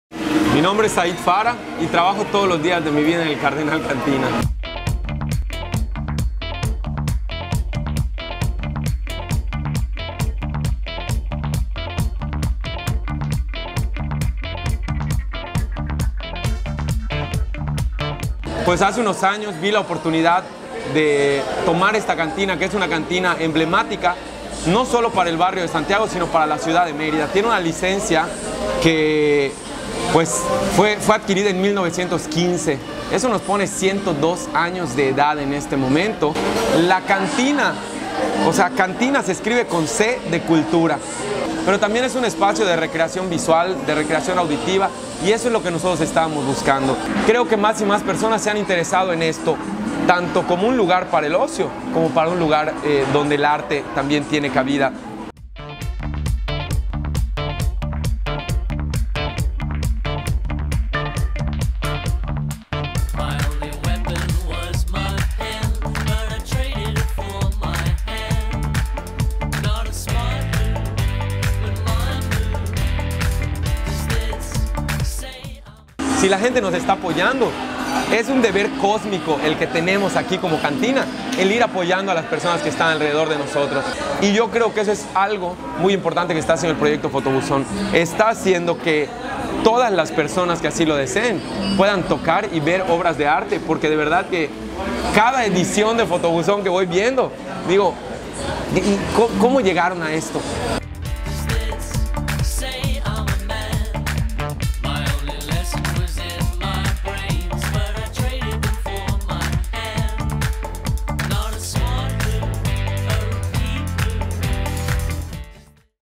FOTOBUZÓN / EL CARDENAL CANTINA
Entrevista con Said Farah Ceh de "El Cardenal Cantina", patrocinador de la edición #30 del proyecto Fotobuzón.

Producción: Gerardo Castillo Herrera / Luis Payán Polanco